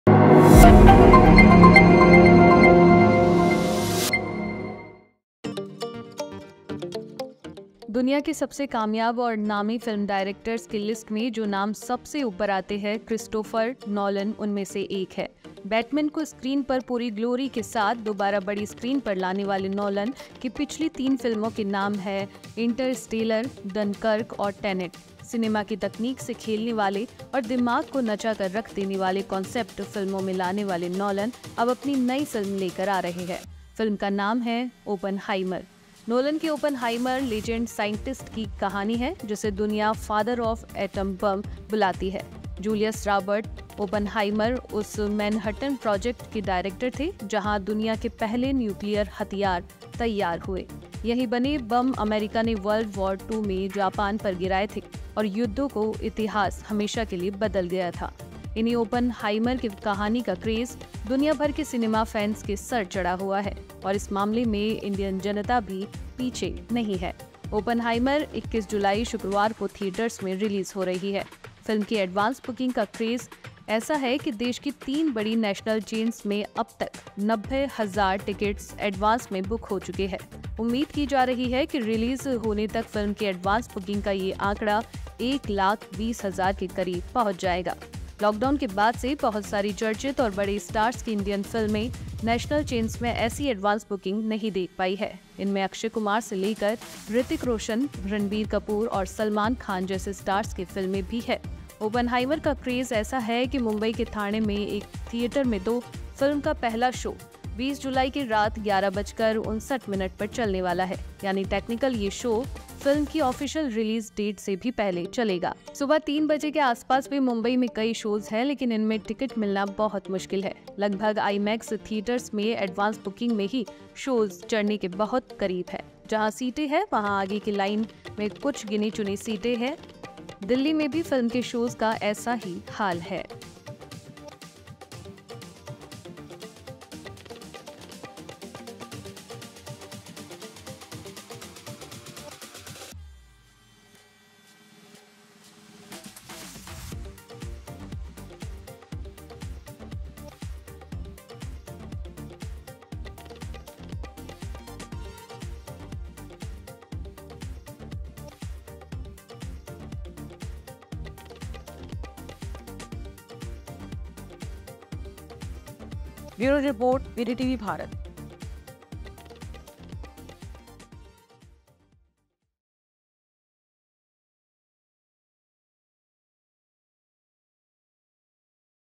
Oppenheimer का जबरदस्त क्रेज,सुबह 3 बजे के Show, 2500 का ticket भी Sold Out | Hollywood |VDTV Bharat
दुनिया के सबसे कामयाब और नामी फिल्म डायरेक्टर्स की लिस्ट में जो नाम सबसे ऊपर आते हैं, क्रिस्टोफर नोलन उनमें से एक हैं. बैटमैन को स्क्रीन पर पूरी ग्लोरी के साथ दोबारा बड़ी स्क्रीन पर लाने वाले नोलन की पिछली तीन फिल्मों के नाम हैं- इंटरस्टेलर, डनकर्क और टेनेट. सिनेमा की तकनीक से खेलने वाले और दिमाग को नचा कर रख देने वाले कॉन्सेप्ट फिल्मों में लाने वाले नोलन अब अपनी नई फिल्म लेकर आ रहे हैं. फिल्म का नाम है 'ओपनहाइमर'. नोलन की 'ओपनहाइमर' लेजेंड साइंटिस्ट की की कहानी है जिसे दुनिया 'फादर ऑफ एटम बम' बुलाती है. जुलियस रॉबर्ट ओपनहाइमर उस 'मैनहट्टन प्रोजेक्ट' के डायरेक्टर थे जहां दुनिया के पहले न्यूक्लियर हथियार तैयार हुए. यहीं बने बम अमेरिका ने वर्ल्ड वॉर 2 में जापान पर गिराए थे और युद्धों का इतिहास हमेशा के लिए बदल गया था. इन्हीं 'ओपनहाइमर' की कहानी का क्रेज दुनिया भर के सिनेमा फैन्स के सर चढ़ा हुआ है. और इस मामले में इंडियन जनता भी पीछे नहीं है. ओपनहाइमर' 21 जुलाई, शुक्रवार को थिएटर्स में रिलीज हो रही है. फिल्म की एडवांस बुकिंग का ऐसा क्रेज है कि देश की तीन बड़ी नेशनल चेन्स में अबतक 90 हजार टिकट्स एडवांस में बुक हो चुके हैं. उम्मीद की जा रही है कि रिलीज होने तक फिल्म की एडवांस बुकिंग का ये आंकड़ा 1 लाख 20 हजार के करीब पहुंच जाएगा. लॉकडाउन के बाद से बहुत सारी चर्चित और बड़े स्टार्स की इंडियन फिल्में, नेशनल चेन्स में ऐसी एडवांस बुकिंग नहीं देख पाई हैं. इनमें अक्षय कुमार से लेकर, ऋतिक रोशन, रणबीर कपूर और सलमान खान जैसे स्टार्स की फिल्में भी हैं. ओपनहाइमर' का क्रेज ऐसा है कि मुंबई के थाणे में एक थिएटर में तो, फिल्म का पहला शो 20 जुलाई (गुरुवार) की रात 11 बजकर 59 मिनट पर चलने वाला है. यानी टेक्निकली ये शो फिल्म की ऑफिशियल रिलीज डेट से भी पहले चलेगा. सुबह 3 बजे के आसपास के भी मुंबई में कई शोज हैं. लेकिन इनमें टिकट मिलना बहुत मुश्किल है. लगभग सभी IMAX थिएटर्स में एडवांस बुकिंग में ही शोज चरने के बहुत करीब हैं. जहां सीटें हैं भी वहां आगे की लाइन मने कुछ गिनी-चुनी सीटें ही अवेलेबल हैं. दिल्ली में भी फिल्म के शोज का ऐसा ही हाल है. मुंबई के ही लोअर परेल इलाके में, एक थिएटर में 'ओपनहाइमर' के लिए 2450 रुपये का भी टिकट है. टैक्स जोड़ने के बाद इस टिकट का दाम 2500 रुपये से ज्यादा हो जाएगा. लेकिन फिल्म का क्रेज ऐसा है कि ढाई हजार से ज्यादा कीमत वाली रिक्लाइनर सीट्स भी सोल्ड-आउट हैं. और ये हाल सिर्फ शुक्रवार के लिए नहीं है. ऑनलाइन चेक करने पर पता चलता है कि इस थिएटर में ये 2500 रुपये से ज्यादा की सीट्स पूरे वीकेंड अवेलेबल नहीं हैं.